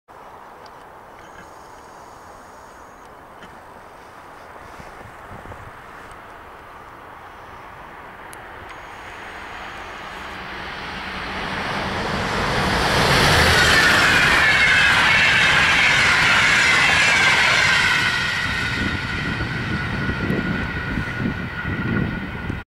Intercity 68 passes Haarajoki
Intercity-juna 68 Kuopiosta Helsinkiin ohittaa Haarajoen lähiliikenneaseman.

Finnish Intercity-train 68 from Kuopio to Helsinki passes Haarajoki commuter station.